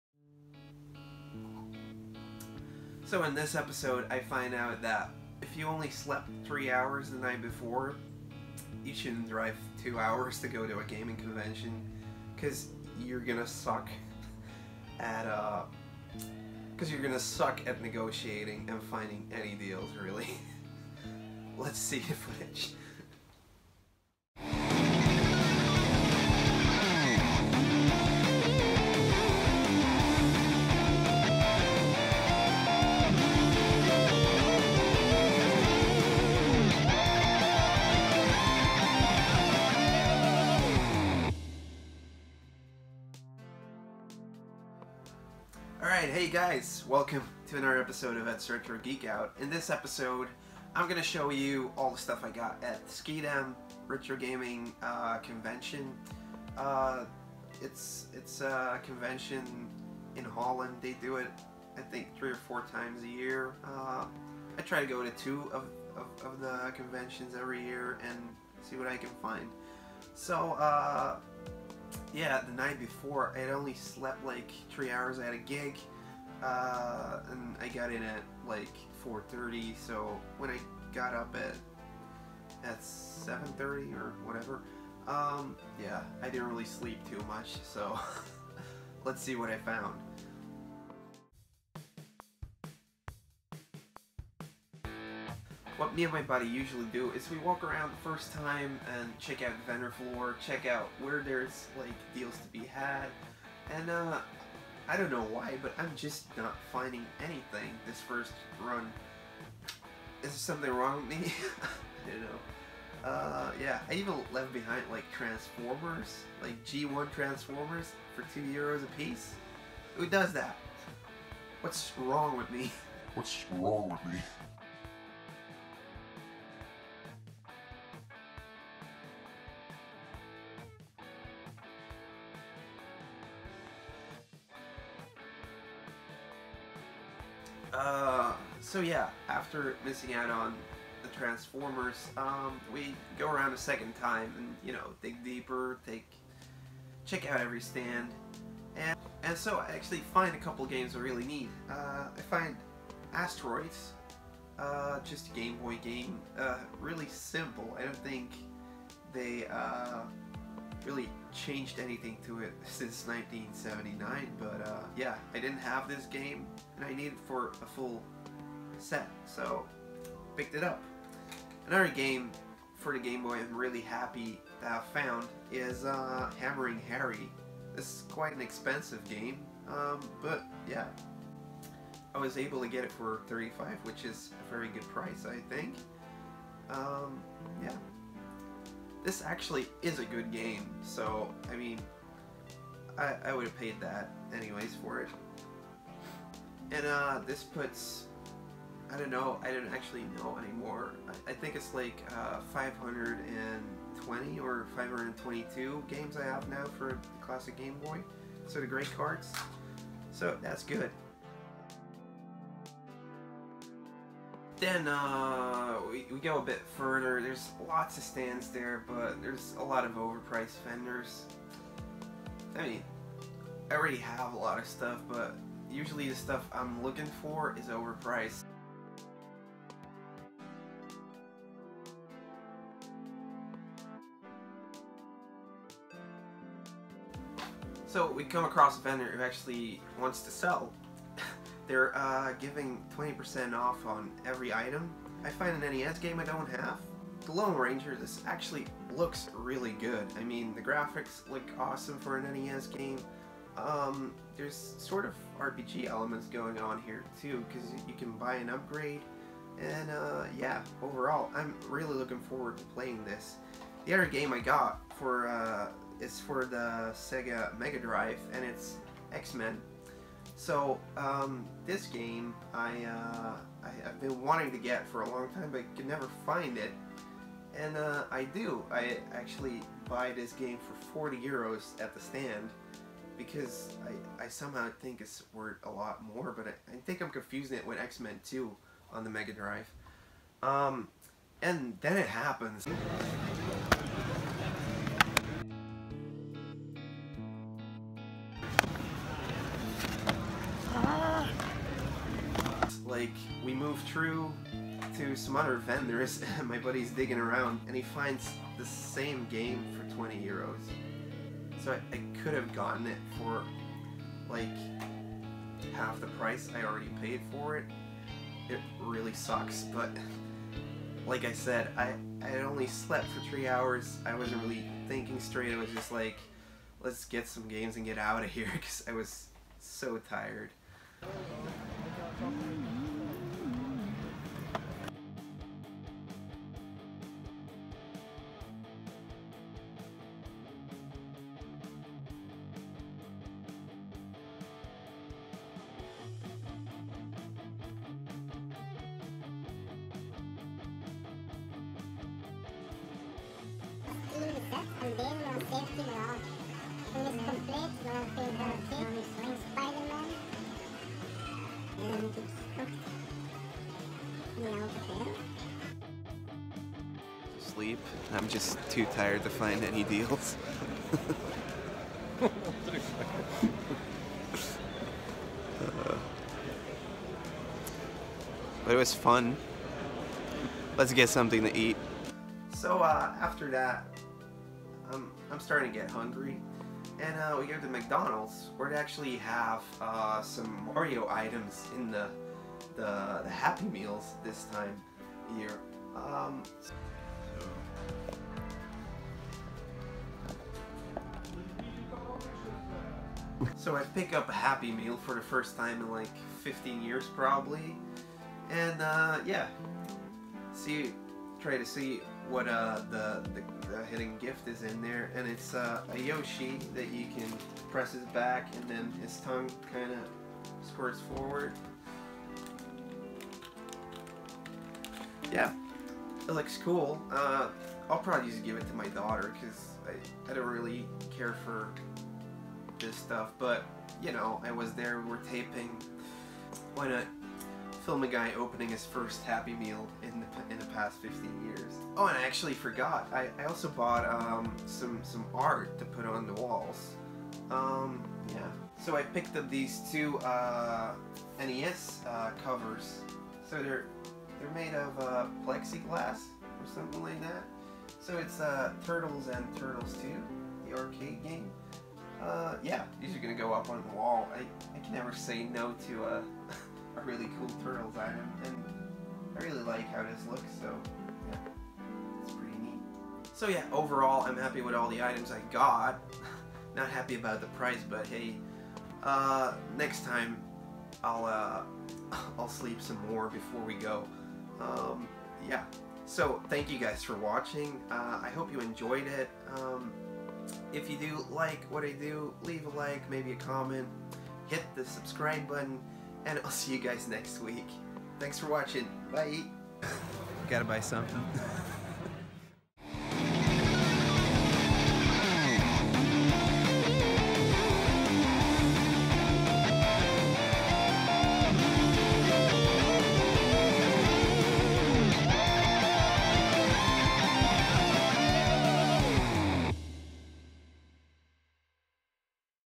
Ed's Retro Geek Out Ep. 5 No Sleep Till Schiedam - Convention Live Pickups
3 hours of sleep and a 4 hour drive make Ed a dull boy!

In this Ed's Retro Geek Out Episode I go gamehunting at the Bonami Convention in Schiedam, Holland. and after drop by Mc Donalds for a much needed Happy Meal and show you all the retrogames I picked up along the way. Among the pickups is Asteroids, Xmen on Megadrive Sega Genesis, Nintendo Gameboy Hammering Harry, NES Lone Ranger and not only do I miss out on a box full of G1 transformers at 2 bucks a piece but I overpay on a game I could have gotten for half the price if only I had looked a bit further. You win some you loose some! Live pickups!

I'm a fan of: The Game Chasers , Grecofabulous , Game Dave , Pat The NES Punk , That Zombie Guy , Secret Gamestash , AVGN , See Jay Are , ...